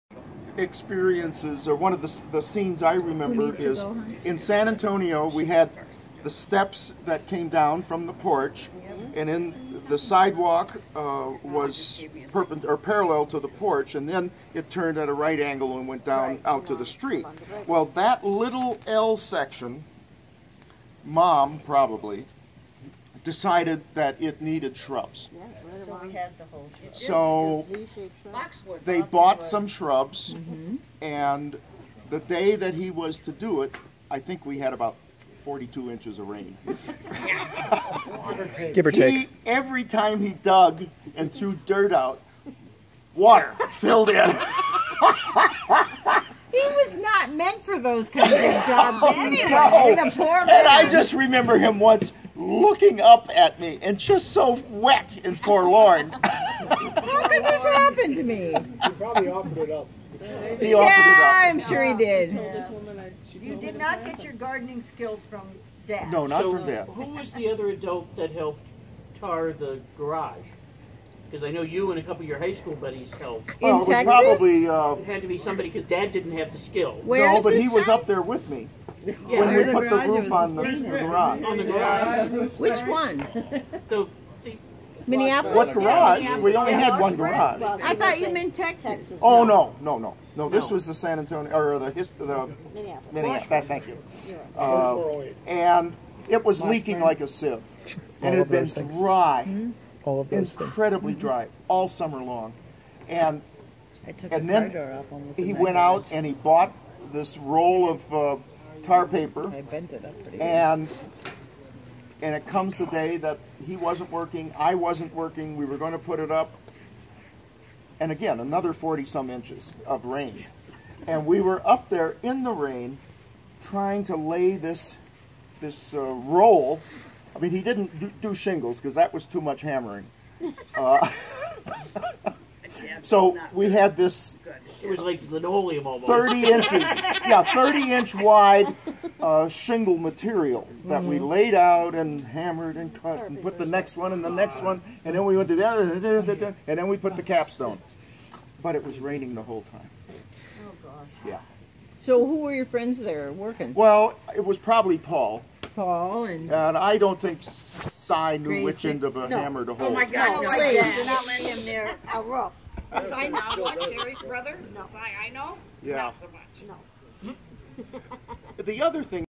Tales of Merlyn: Planting and roofing
The extended Coleman family gathered July 28, 2017 to celebrate the 100th birthday of Merlyn Coleman and share remembrances. These are their stories.